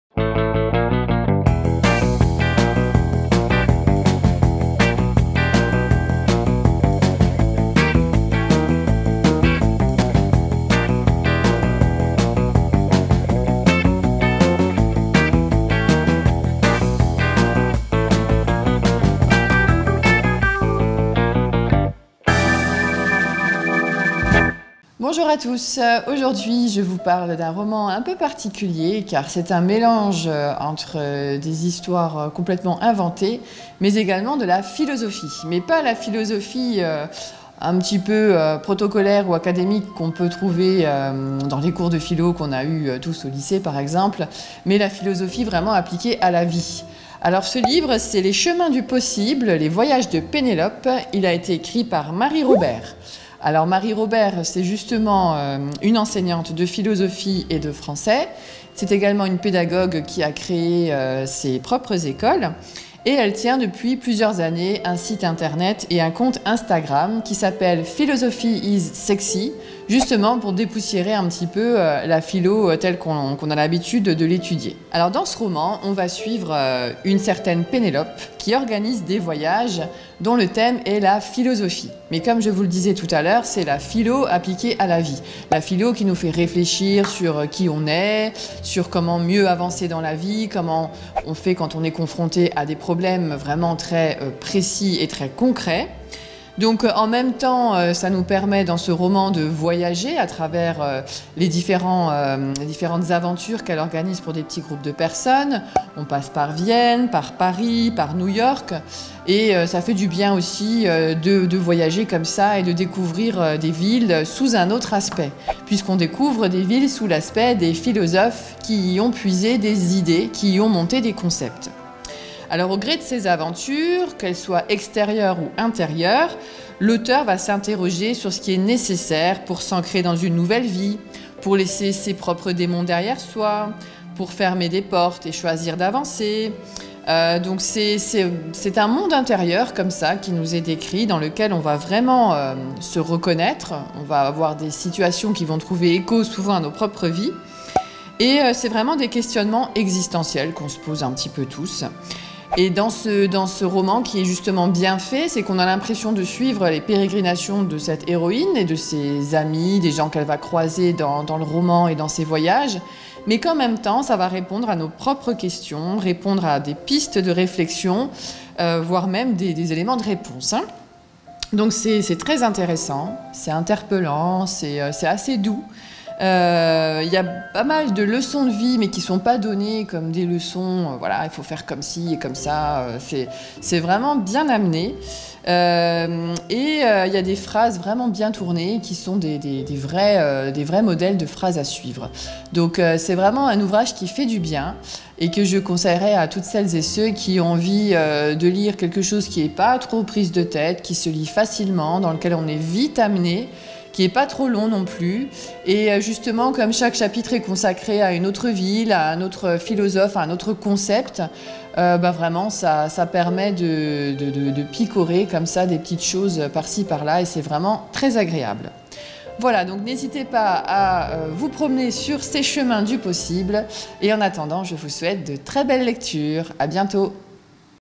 Et toi, tu lis quoi ? "Les chemins du possible - Le voyage de Pénélope" de Marie Robert
Aujourd'hui dans "Et toi, tu lis quoi ?", Alexandra vous présente  Le voyage de Pénélope" de Marie Robert.
"Tu veux l'intensité sans le risque. Tu veux la vie sans la mort. Tu veux le sublime sans la tempête. Tu veux l'amour sans le quotidien. Il y a cinq ans, tu as fui pour aller au-devant de toi-même, pour t'émanciper de tes carcans, et bonne nouvelle, tu as réussi. Mais maintenant, c'est à toi d'inventer une existence qui te convient. Entretenir la douleur est une autre manière de fuir. Il est temps de faire la paix avec tes fantômes et de trouver ton port."Ceci est une épopée. Entre Boston, Vienne, Paris, Genève, São Paulo... "